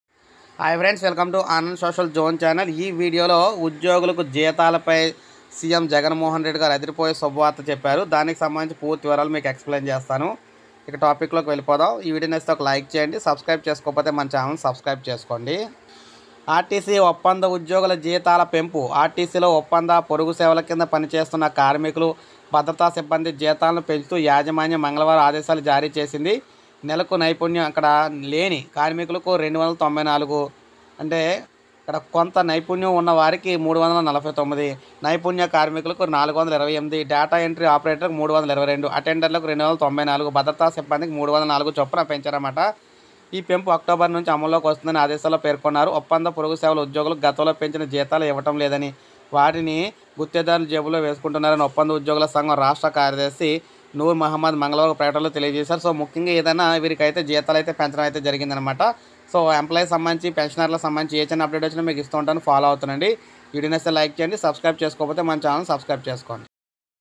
ఉద్యోగుల జీతాల పెంపుపై సీఎం జగన్ సంచలన ప్రకటన employees salaries latest update in AP.
Hello everyone today update is AP government announced that employees salaries latest update today .@Anandsocialzone